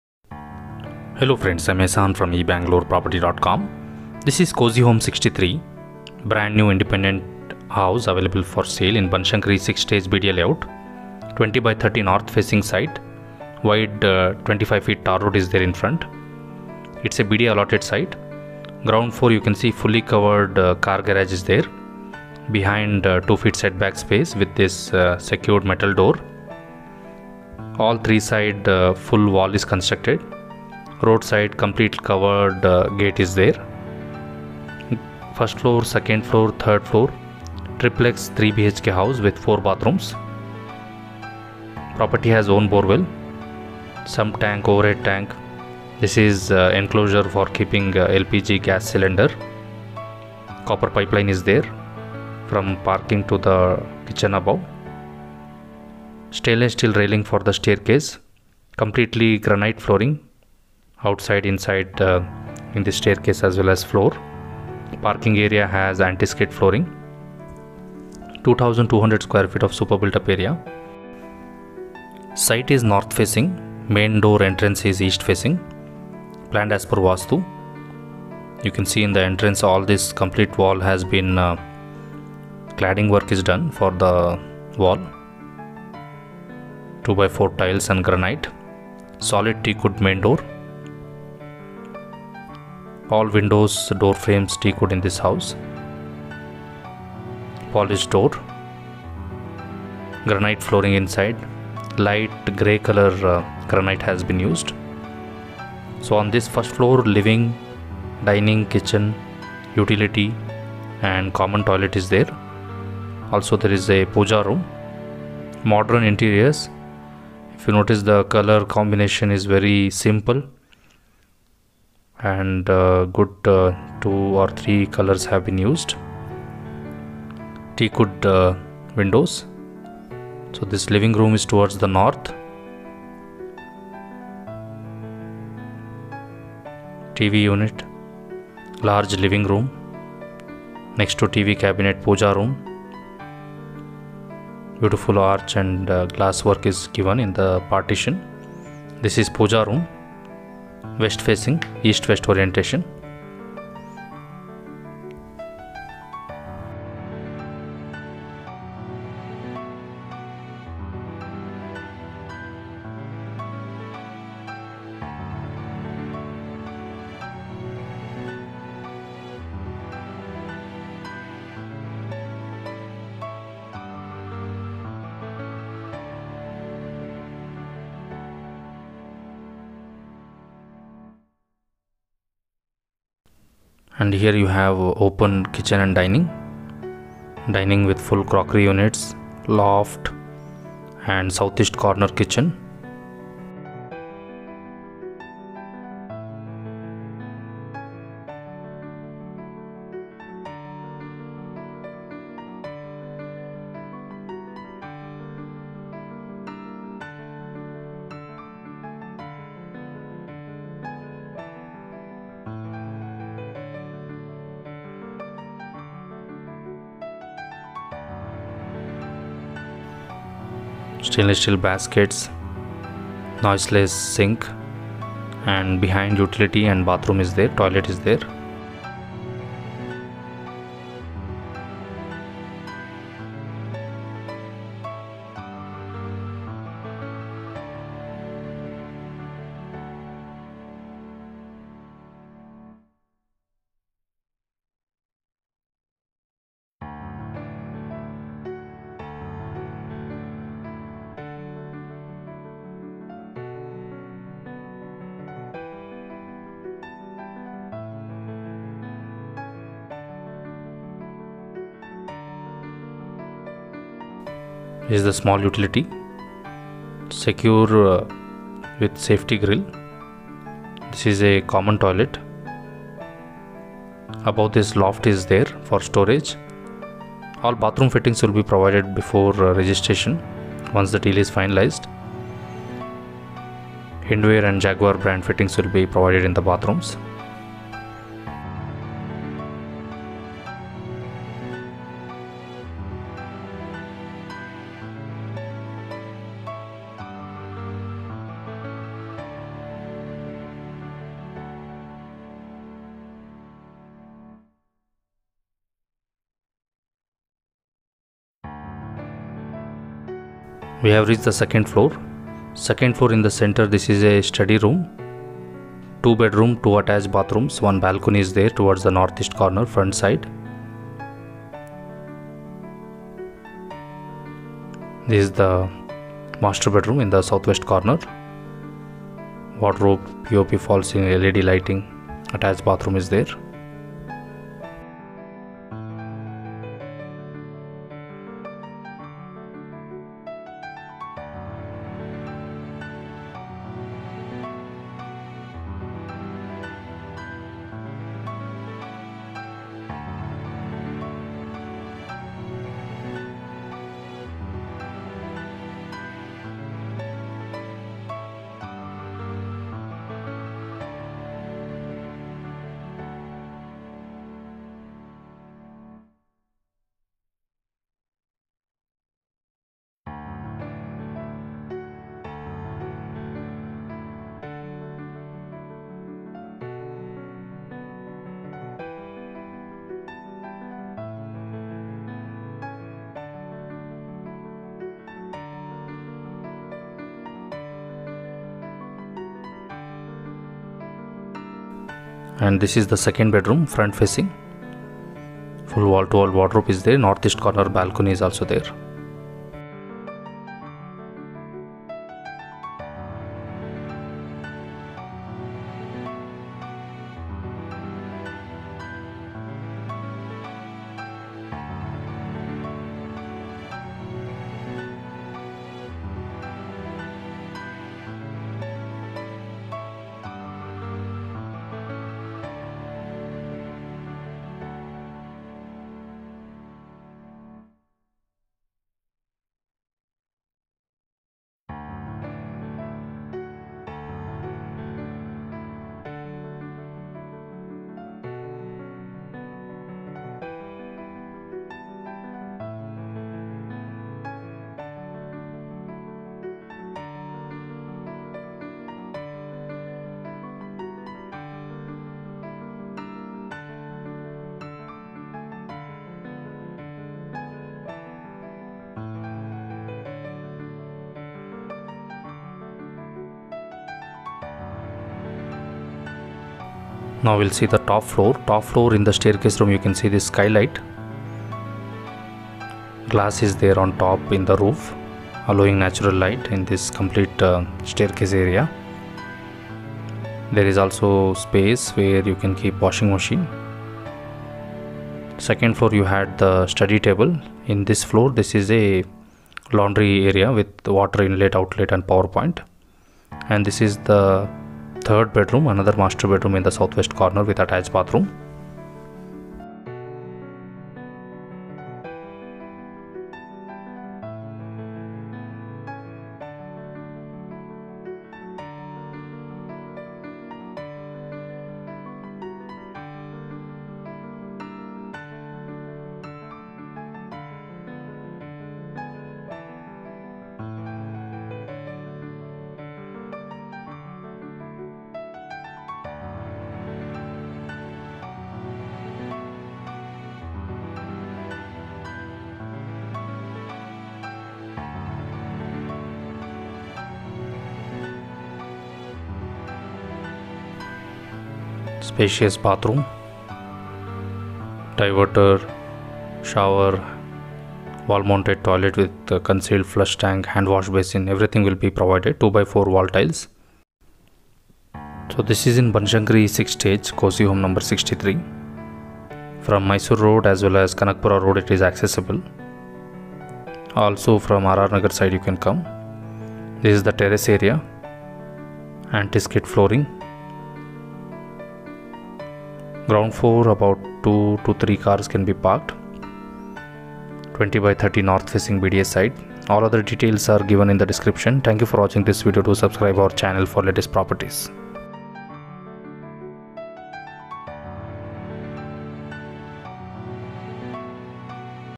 North Facing 3BHK Triplex New Home For Sale BSK6 BDA
Banashankari 6th Stage BDA Layout off Mysore Road Bangalore, Brand New North Facing Independent 3BHK Cozy Home No.63 For Sale.

BDA North Facing A Khata Site 20x30(600 Sq.ft),  East Facing Main Door, 2200 sft SBU Area, Planned as Per Vaastu, Granite Flooring, Ground floor 3 Car Parking, 1/2/3 Floor Triplex 3BHK House with 4  Bathrooms, Own Borewell ,Tar Road, Developed Locality.

Above Cozy Home No.63 is Priced at Rs.115 Lakhs Negotiable.

Kindly Watch and Listen to Complete Video Read the Full Description then Call- for further Information and sitevisit.

Bangalore, Karnataka, India. Monday, 10th October, 2022. English Audio.